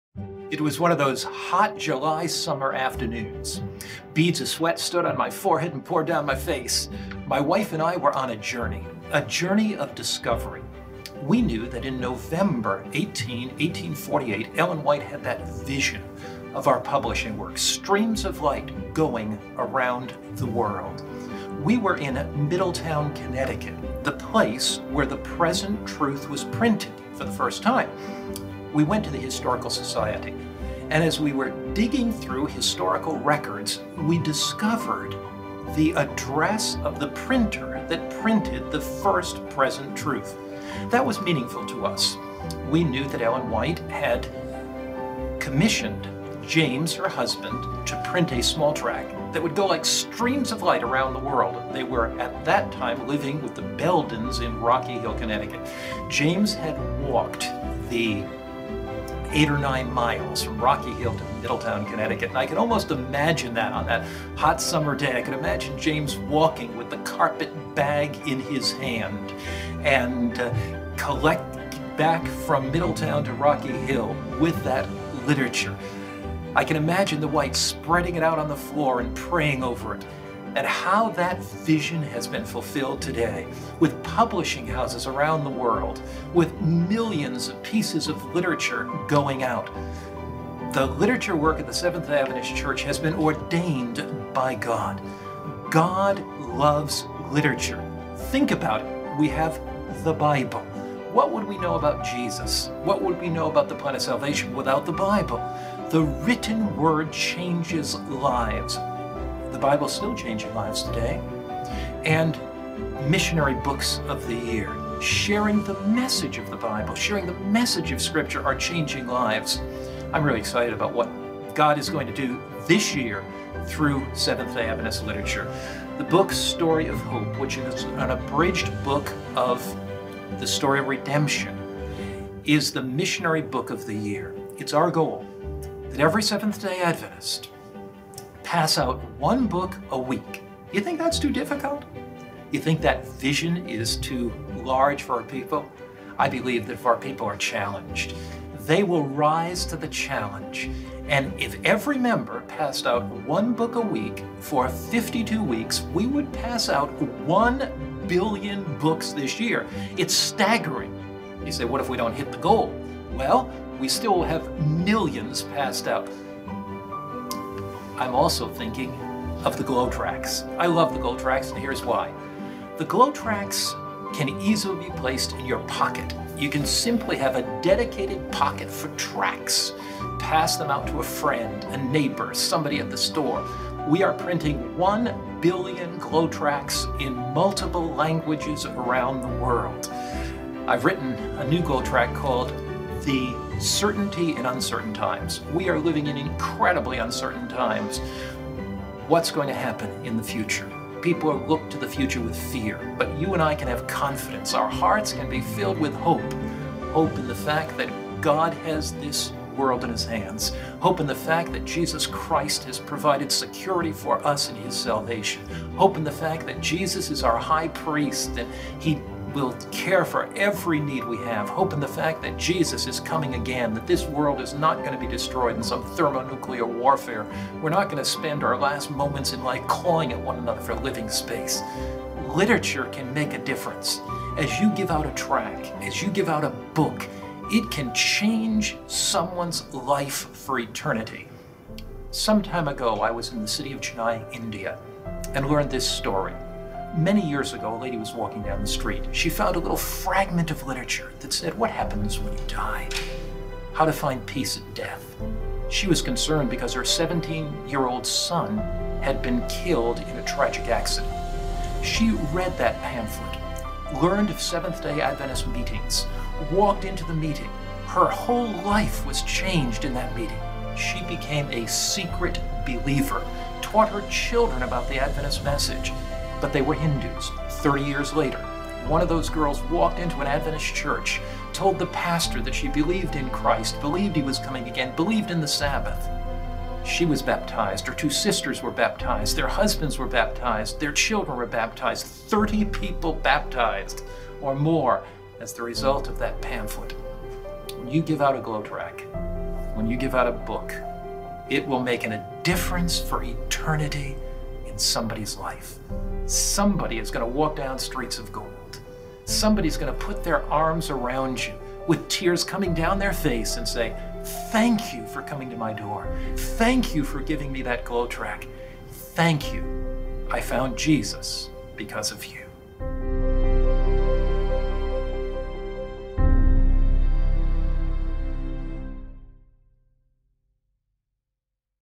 Mark Finley – Story of Hope – Mission 365
Mark Finley shares Ellen White’s November 1848 vision that Adventist Publishing work would go around the world like streams of light. Today, you can send these “streams of light” around the world by giving those you come into contact with the 2017 Missionary Book of the Year – The Story of Hope.

The Story of Hope is an abridged version Ellen White’s classic, The Story of Redemption – the core message of Conflict of the Ages set – the story of good versus evil.

Mark Finley challenges Seventh-day Adventist church members to share one copy of this book each week with someone.

The Story of Hope is available on AdventistBookCenter.com.